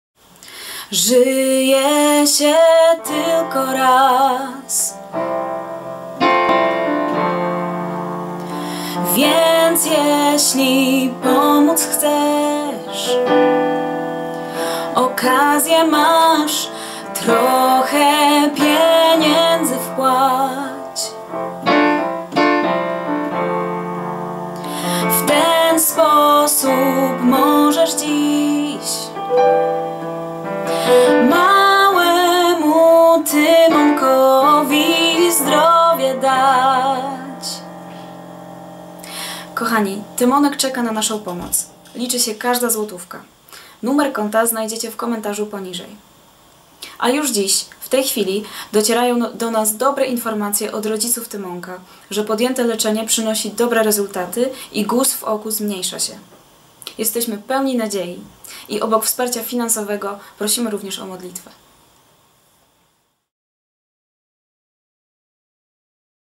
Gabriela Gąsior zachęca do włączenia się do akcji pomocy dla Tymonka - Puk puk Tymonku
Gabriela Gąsior zachęca do włączenia się do akcji pomocy dla Tymonka Palczarskiego.

- SMS o treści PUK PUK na nr. 74567 koszt: 4,92 brutto w tym VAT
- SMS o treści PUK PUK na nr. 79567 koszt: 11,07 brutto w tym VAT
- Tradycyjnie przelewem:
* Na konto FUNDACJA RYCERZE I KSIĘŻNICZKI, UL. ZAGÓJSKA 2/4/85, 04-101 Warszawa
tytułując przelew „DAROWIZNA DLA TYMONA"
Na rachunek: PKO Bank Polski SA, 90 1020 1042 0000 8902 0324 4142,
Dane do wpłat zagranicznych:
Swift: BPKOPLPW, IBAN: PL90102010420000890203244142
Konto walutowe USD:
IBAN: PL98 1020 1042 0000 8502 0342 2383 .
Zapraszam również na:
*Musical.ly:  swiat_wedlug_sabinki
*Profil kanału "Świat według Sabinki" na Facebooku: